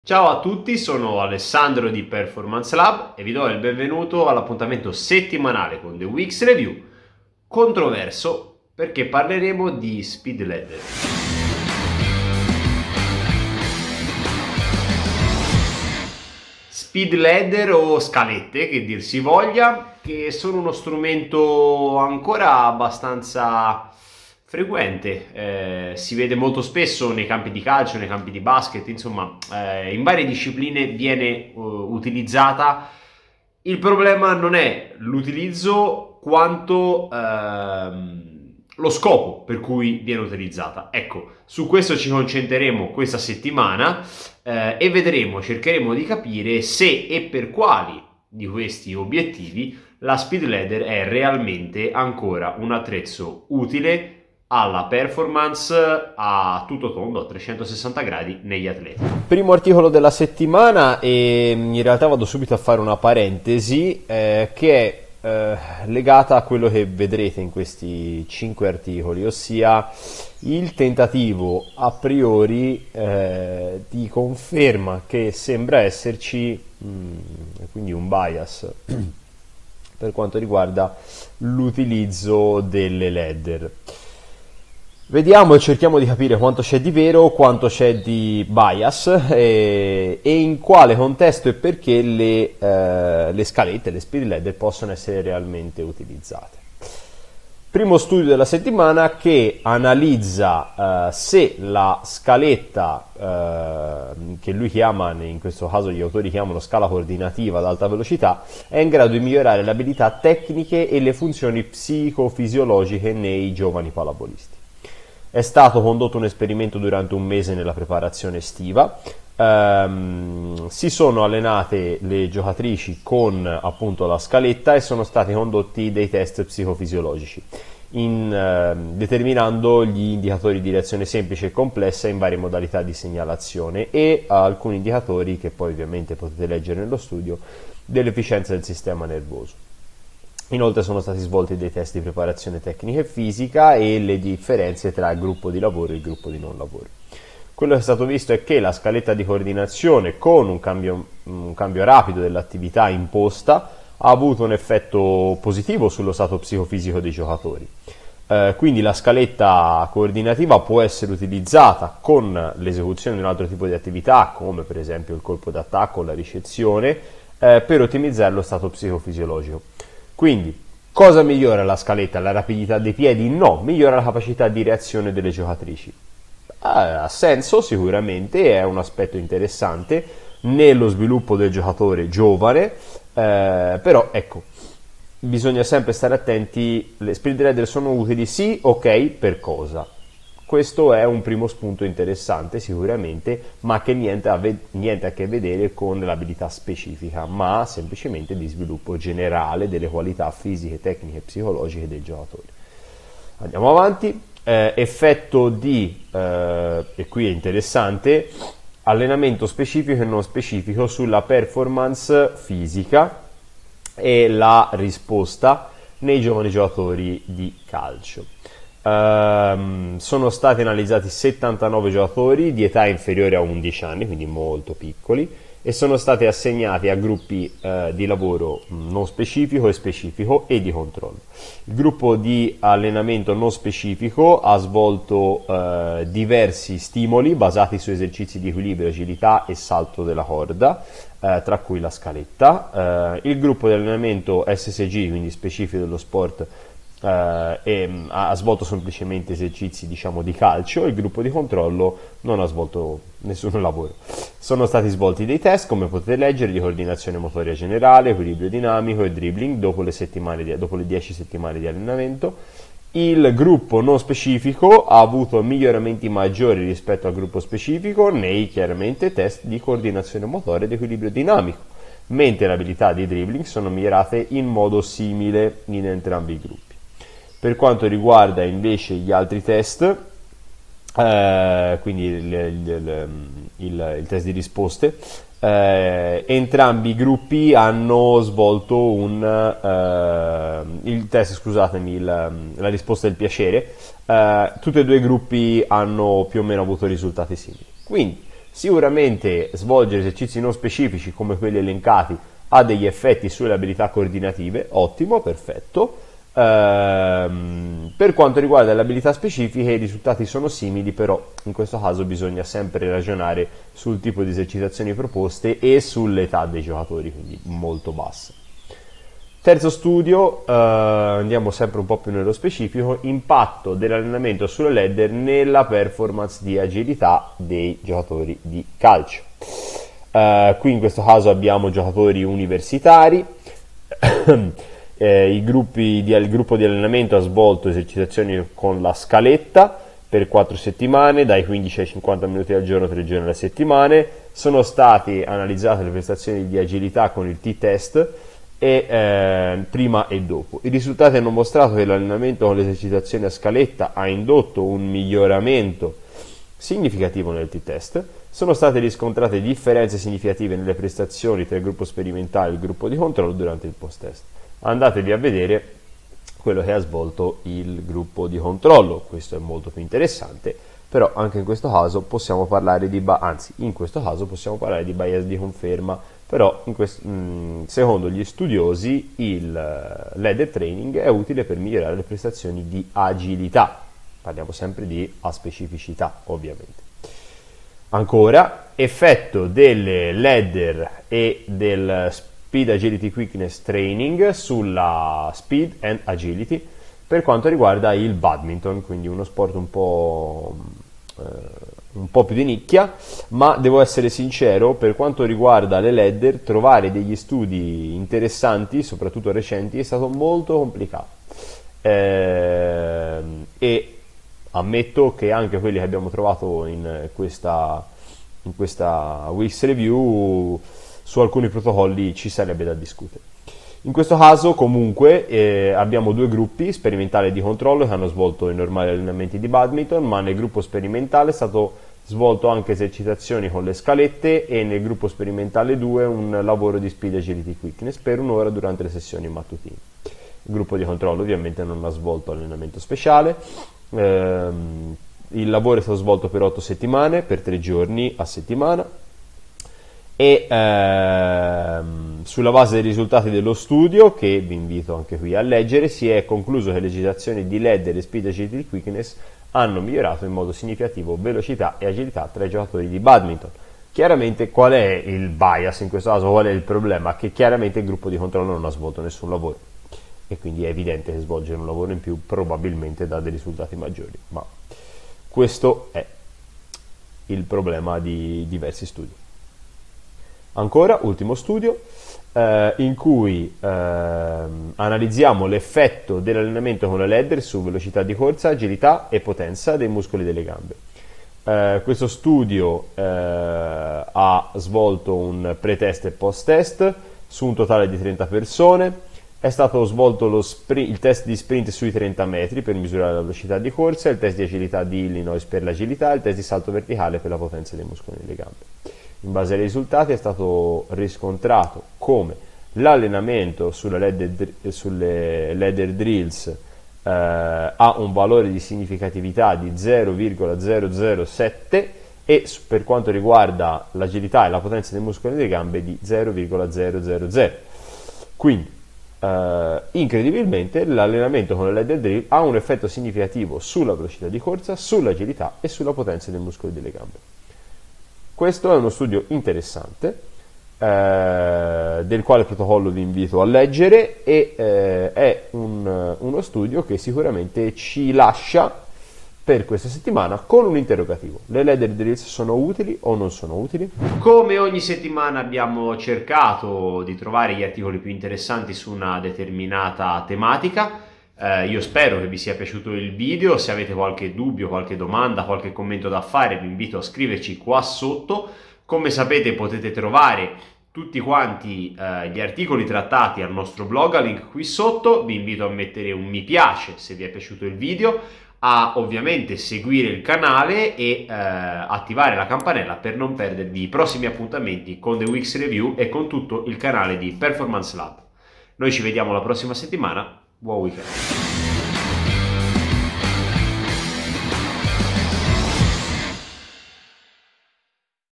Speed ladder: si o no?
Le speed ladders, o scalette, sono ancora oggi utilizzate dai preparatori fisici in diversi contesti sportivi. Nonostante la loro utilità sia stata messa in discussione, riscuotono tutt'oggi un discreto successo.

La speed ladder è davvero un mezzo di allenamento utile al miglioramento delle qualità fisiche degli atleti? E se si, quali, e in che modo?

Settimana di "The Week's Review" dedicata alla speed ladder: validità e strategie di utilizzo. Di cosa abbiamo parlato nell’analisi degli articoli settimanali?

00:00 Introduzione
01:00 Ladder e qualità psicofisiche
03:21 Allenamento a-specifico vs specifico
05:19 Scalette nel calcio?
06:27 Ladders drill e SAQ
08:00 Velocità, agilità e potenza con le ladder
09:14  Buona lettura sul nostro blog!

Quali sono le tue idee, avevi già letto questi articoli?

Vuoi provare GRATIS la piattaforma di Performance Lab? Registrati ora

Avrai accesso subito ad una selezione di contenuti 🔥